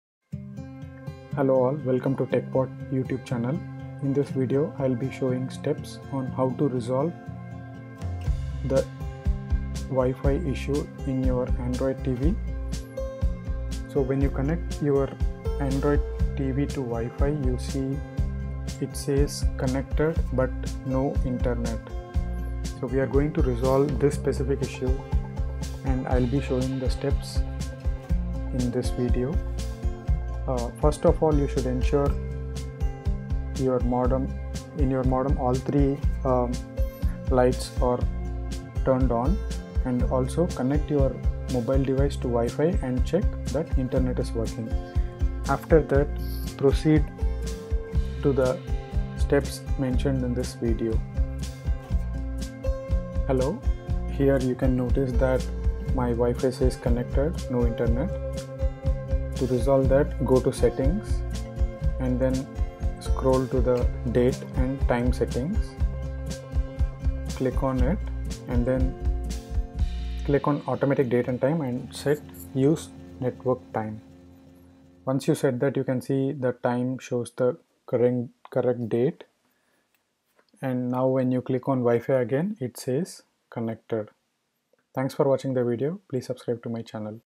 [Solved] Android TV Connected, no Internet problem
Simple and easy fix for "Connected, no Internet" problem in your Android TV - Sony, Samsung, LG, Mi etc.

Follow the steps in this video to fix the problem